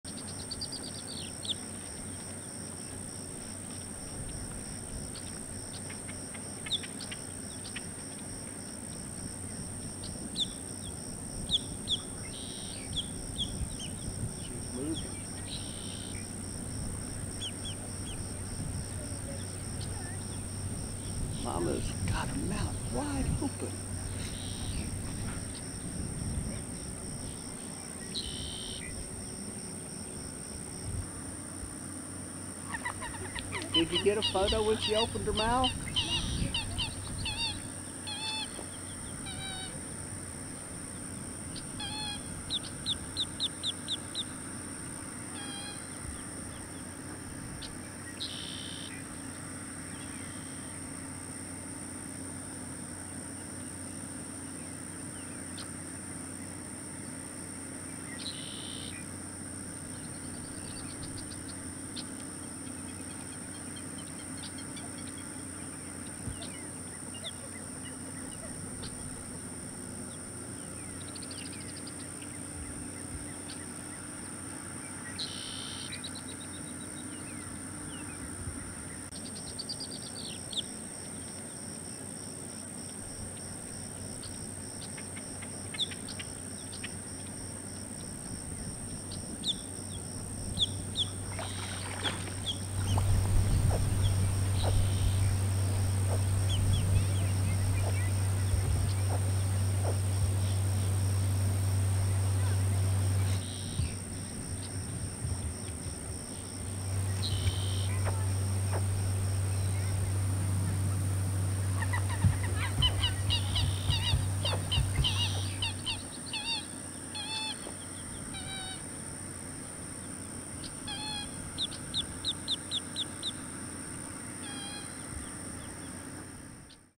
Baby Gators Chirping
American Alligator sow with several hatchlings at Circle B Bar Reserve located just outside of Lakeland Florida. The hatchlings begin their characteristic chirping partway through the video. Toward the end, Mom picks one or two up in her mouth and carries them away. After first hatching out of the eggs, the sow picks the hatchlings up and carries them from the nest to the water. Here is one of those instances where the sow carries hatchlings a while after hatching.